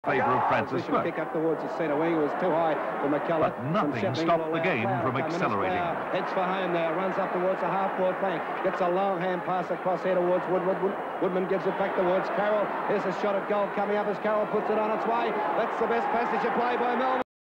The Collingwood Player Archive: Russell Jessop
Russell Jessop played for Collingwood in 1976 and 1977, appearing in 5 games. Russell was a versatile player who played in a number of positions, despite being most suited to the forward line. In this footage from Round 22 1976, he can be seen wearing number 49. Despite his brief career in the seniors, Russell managed to play in a premiership with the Under 19's in 1974 and the Reserves in 1976. Sadly Russell passed away in 2018. For more information about his career at Collingwood, go